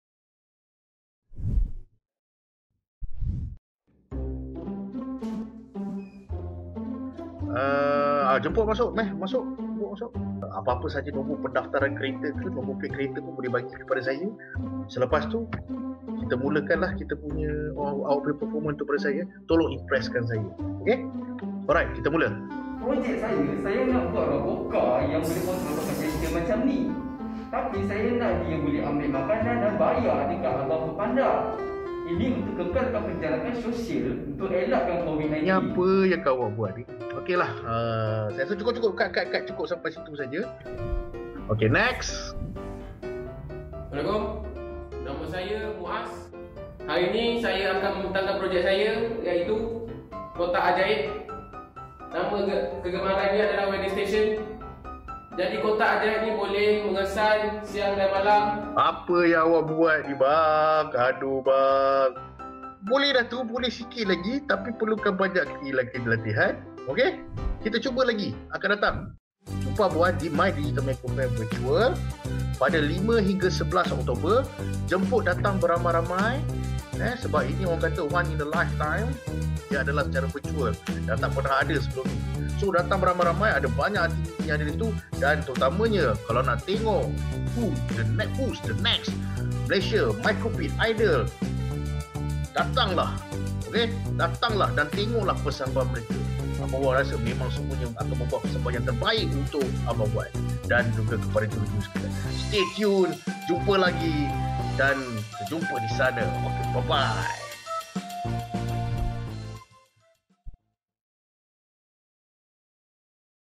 Road to Malaysia's Microbit Idol 2020 - mydigitalmaker Virtual Fair 2020
Boleh percaya ke Semi-Finals Road to Malaysia's Microbit Idol sudahpun dekat? Sertailah kumpulan Elvira Systems Sdn Bhd di mydigitalmaker Virtual Fair pada 5-11 Oktober 2020 untuk pertempuran terakhir dan ketahui siapa yang akan menjadi Microbit Idol pertama di Malaysia!!
Jangan terlepas! Jumpa anda di sana!